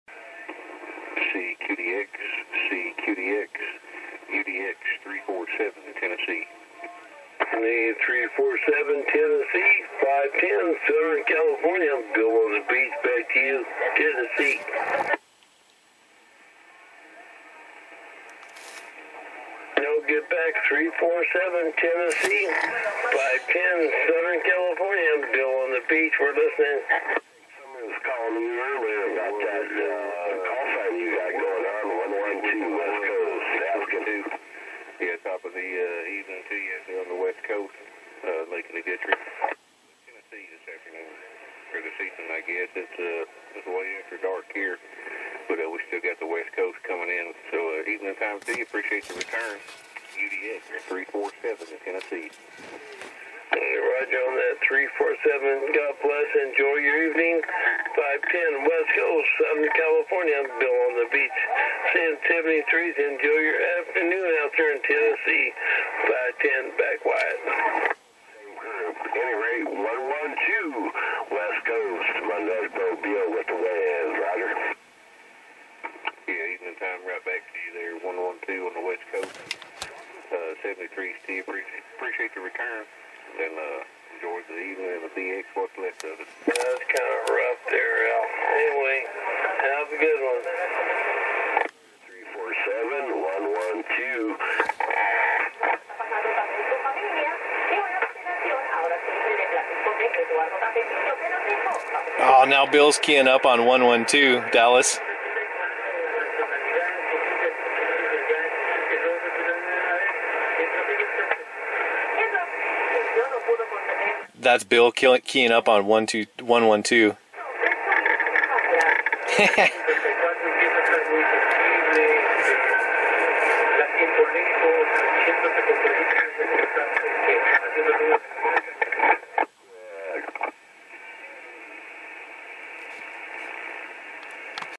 510 poacher Bill not happy
Looks like 510 got upset that 112 got through and he didn't so at the end he has a bit of a tantrum and tries to dead key Spanish stuff over 112. This is what we deal with out here. 510 Bill the poacher does it to everyone local.